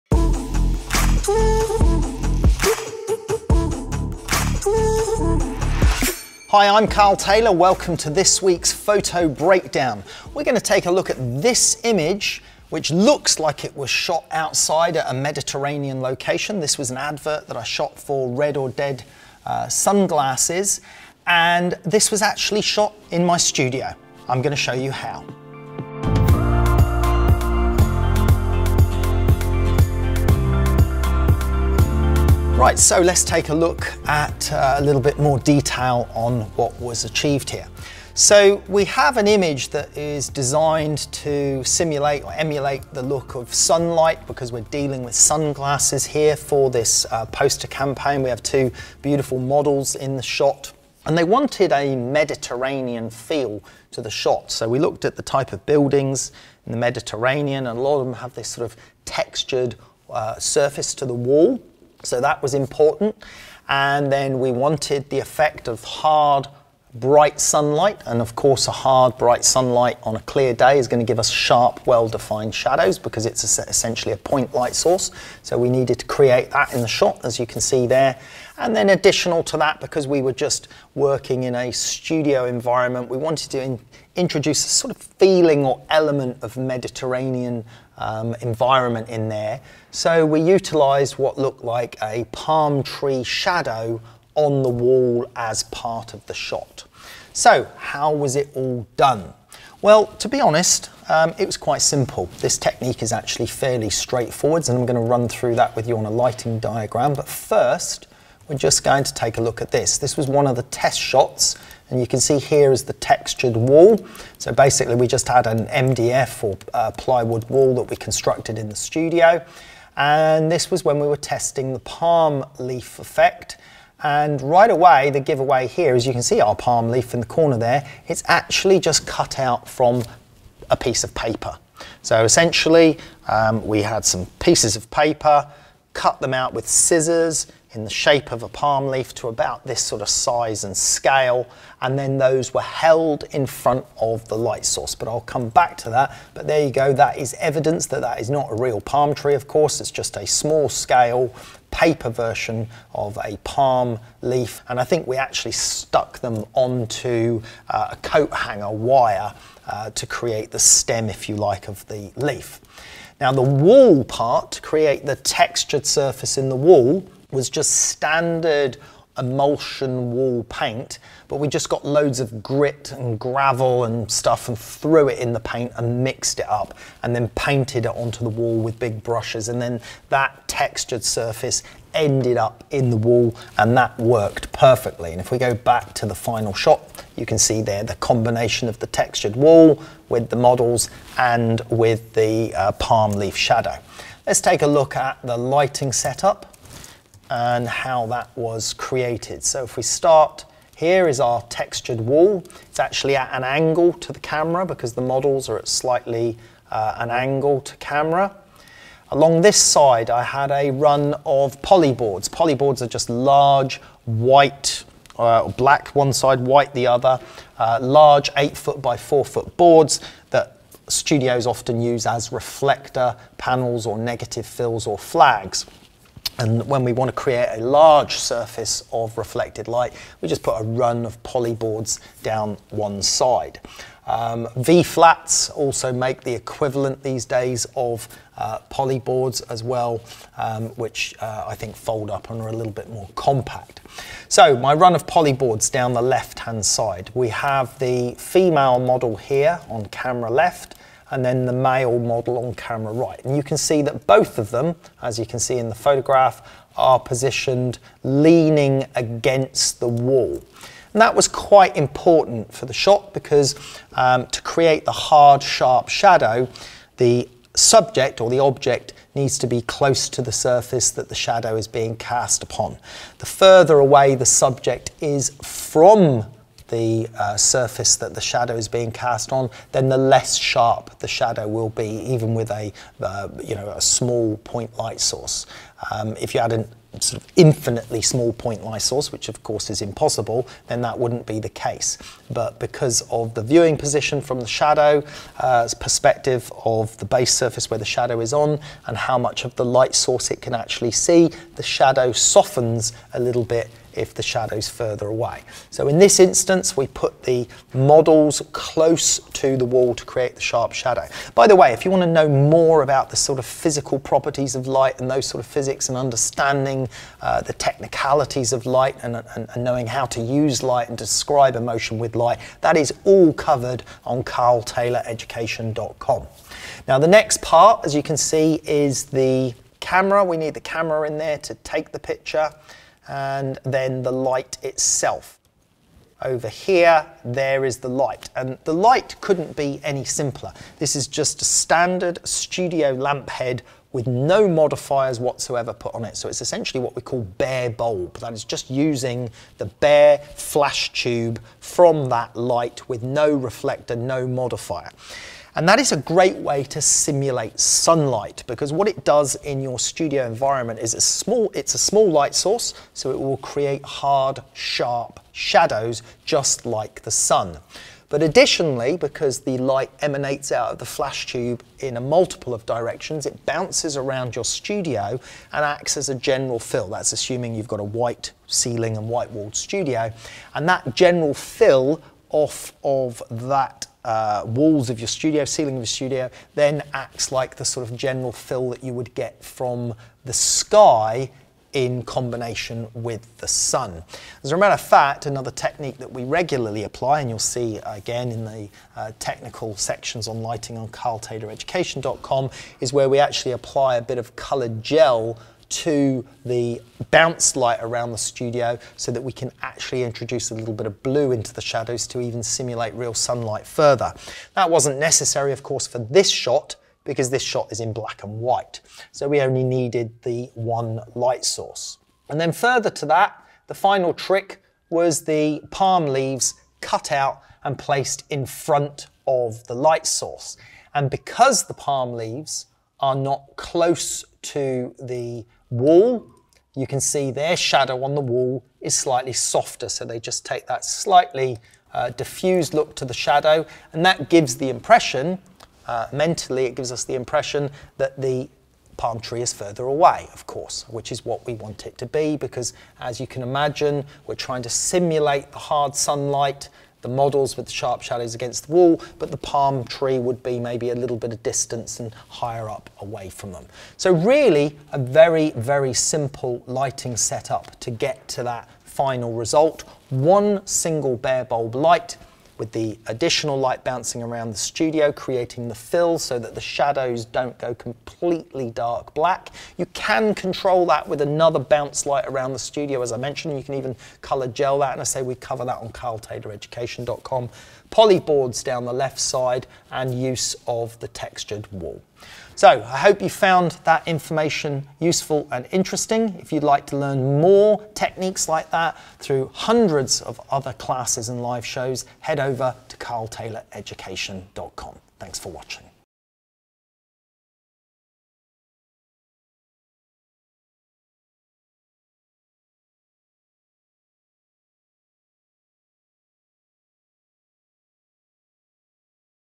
Studio Sunlight Photography: Mediterranean Vibes
For this sunglasses campaign, the client wanted a Mediterranean vibe. So naturally I jetted off to the Med to shoot it on location.

Or did I? In fact, this photoshoot took place in my studio – and those weren’t even real palm fronds!

Check out the video to discover how I created that hard imitation sunlight, the Mediterranean-style textured wall, and of course those palm frond shadows. Here’s a clue: it involved a coat hanger, a pair of scissors, and some plain white paper.

Fashions may change over the years, but effective photography techniques never go out of style.

📸  Accelerate your progress with hundreds of classes, support, and perks!